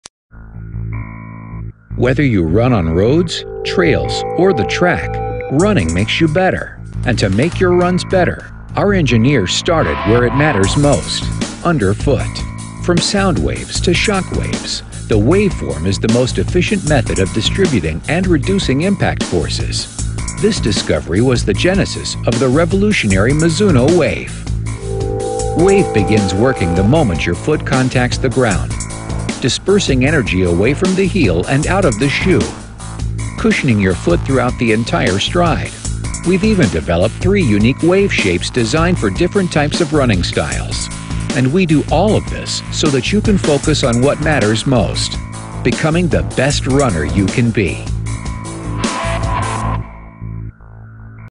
Footwear Explained: Mizuno 'Wave Technology'
Mizuno are renowned for quality sports products... so what's the thinking behind the 'Wave'?
Video, with thanks, Mizuno.